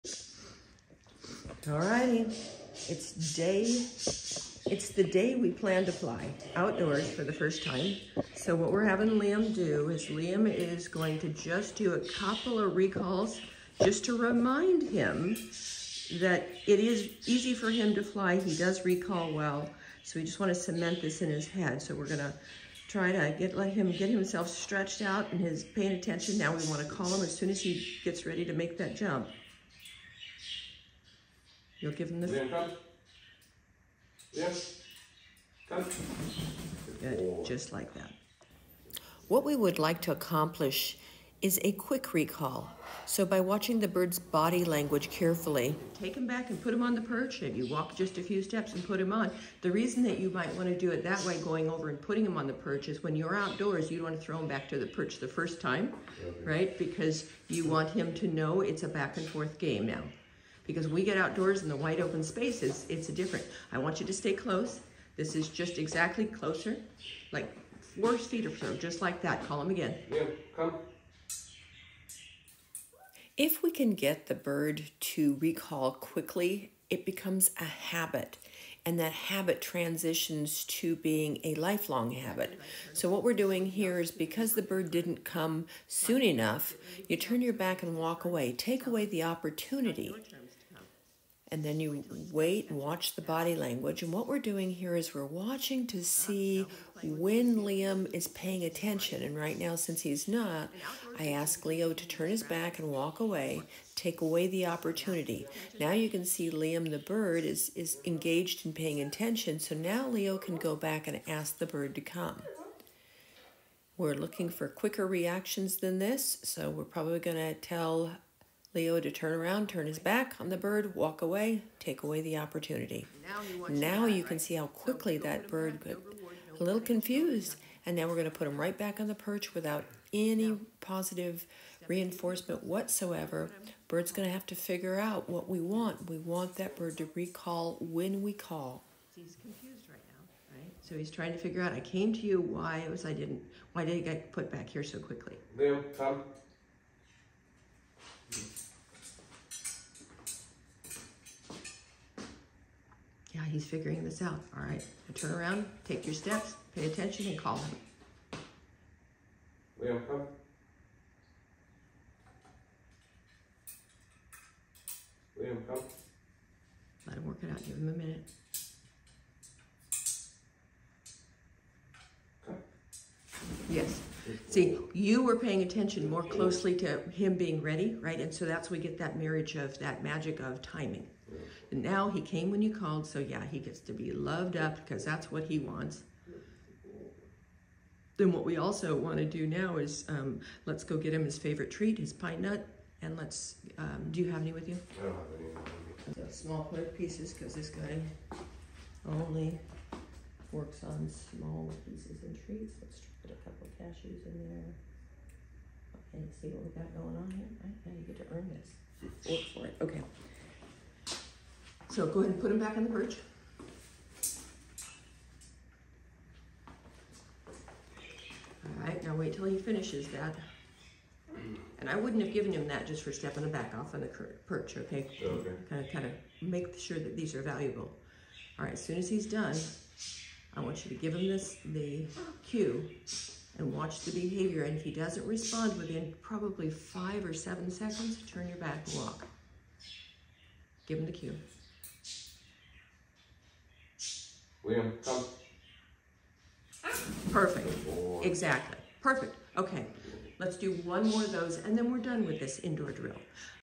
Indoor Recall with Macaw
short video illustrating how we train for recall